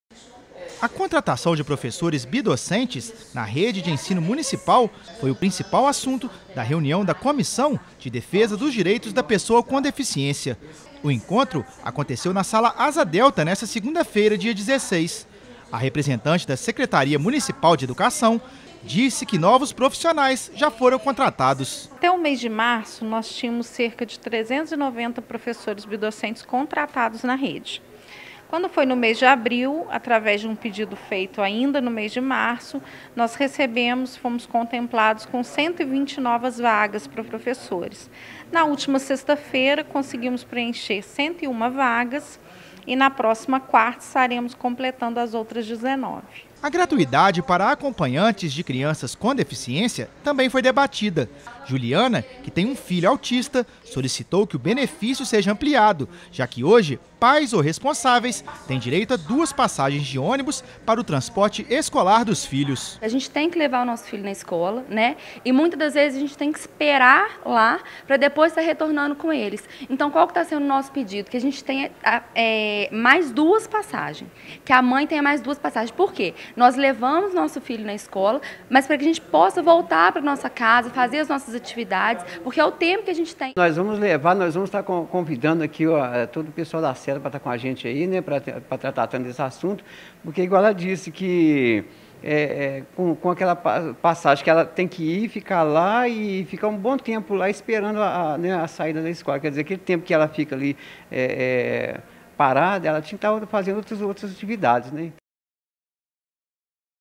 Comissão debate contratação de professores bidocentes
A Comissão de Defesa dos Direitos da Pessoa com Deficiência se reuniu na Câmara Municipal nesta segunda-feira, 16. Outro tema discutido foi o direito dos pais de crianças com deficiência terem direito a quatro passagens no transporte público.